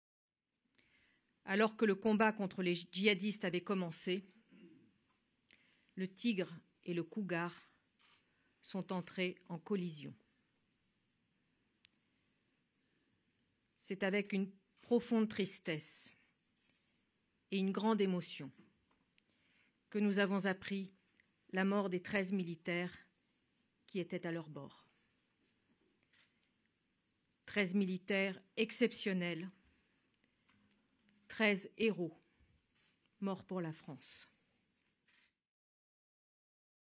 Soldats tués au Mali: "13 héros morts pour la France" (Parly) | AFP Extrait
La ministre des Armées Florence Parly rend hommage aux treize militaires morts dans un accident d'hélicoptère au Mali lundi 25 novembre, des "hommes exceptionnels, treize héros morts pour la France."